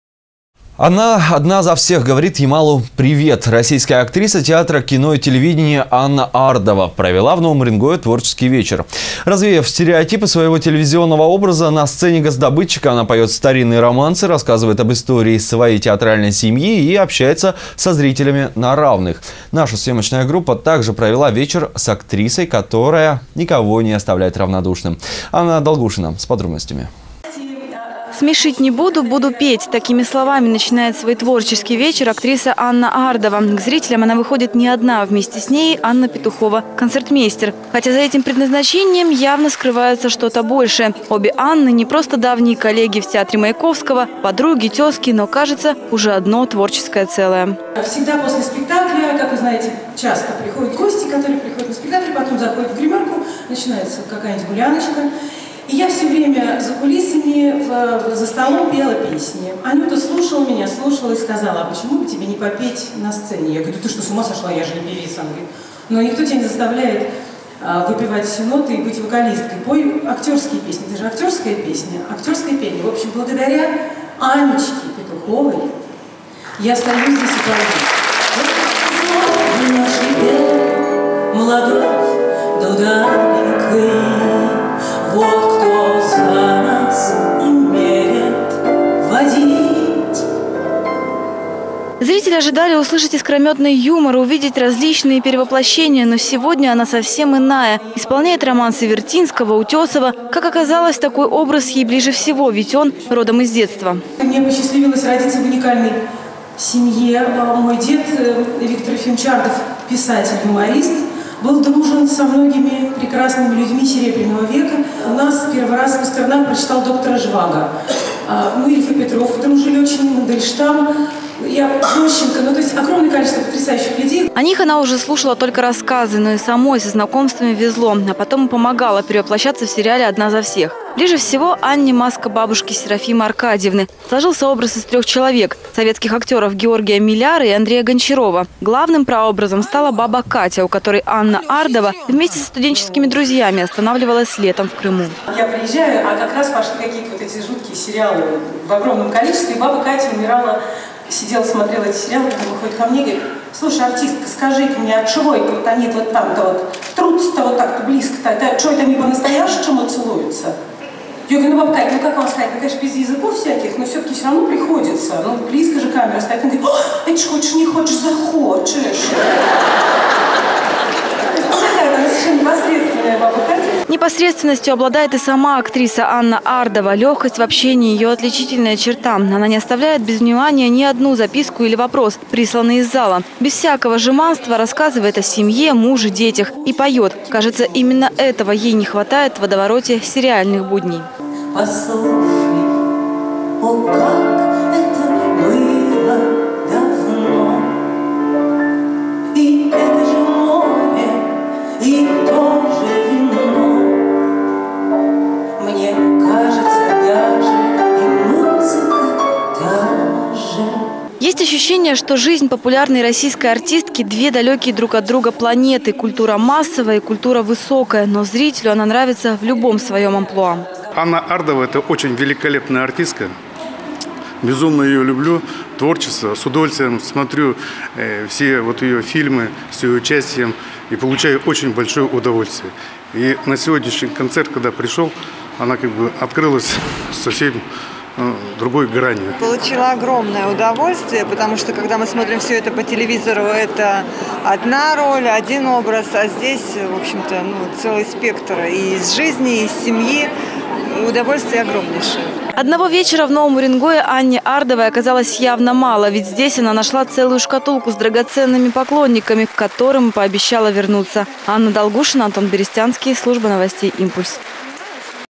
Анна Ардова провела творческий вечер в Новом Уренгое.
Она одна за всех говорит Ямалу «привет». Российская актриса театра, кино и телевидения Анна Ардова провела в Новом Уренгое творческий вечер. Развеяв стереотипы своего телевизионного образа, на сцене «Газодобытчика» она поет старинные романсы, рассказывает об истории своей театральной семьи и общается со зрителями «на равных». Наша съемочная группа также провела вечер с актрисой, которая никого не оставляет равнодушным. Анна Долгушина с подробностями.